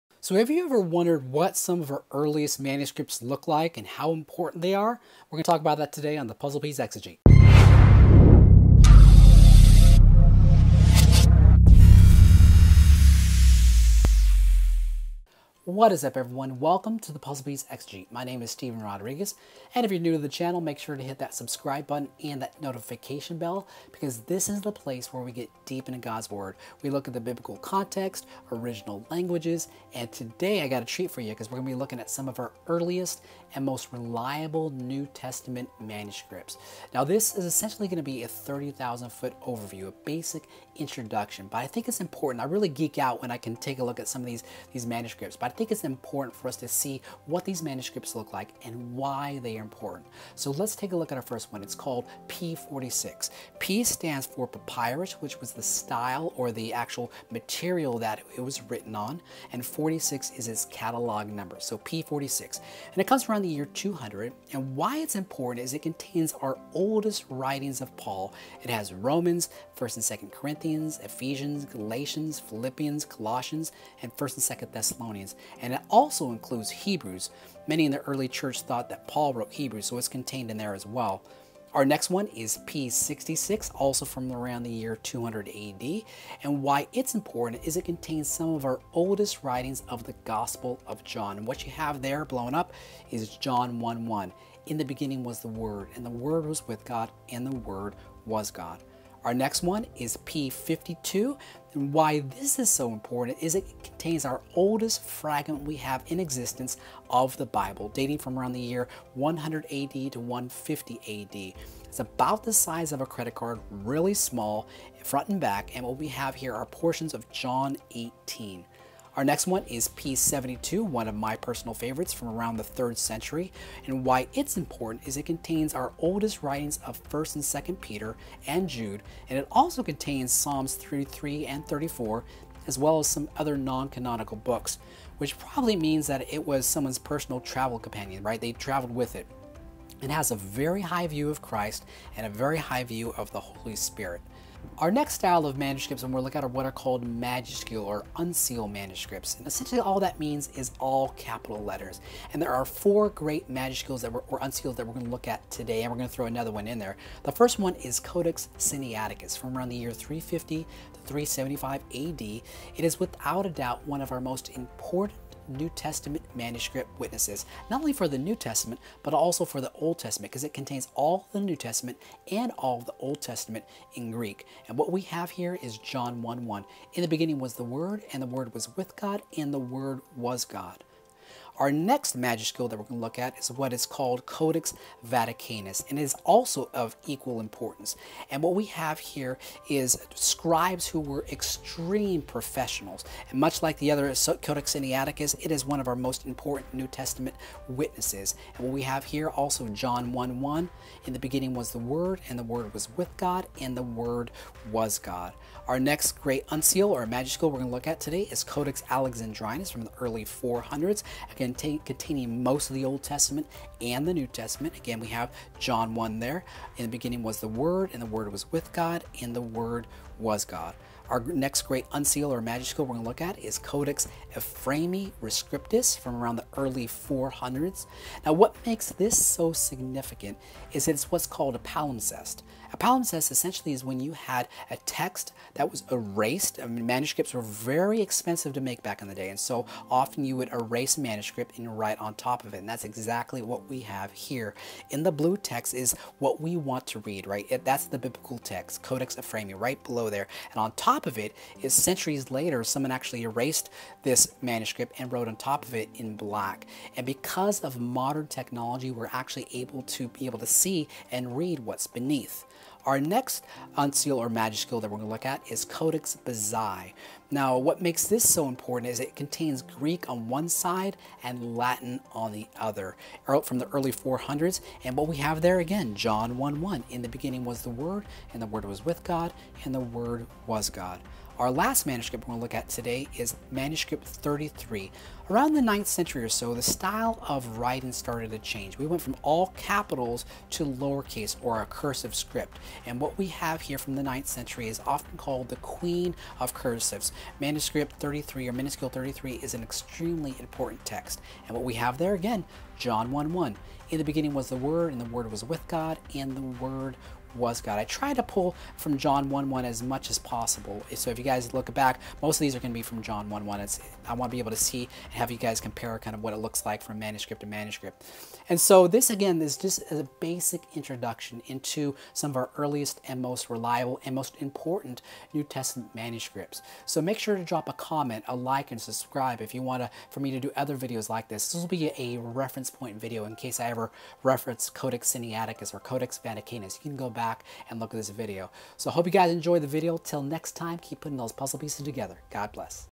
What Are Our Earliest Manuscripts of the New Testament?
In this video, we look at some of our oldest and most important manuscripts of the New Testament. This is a basic overview and introduction.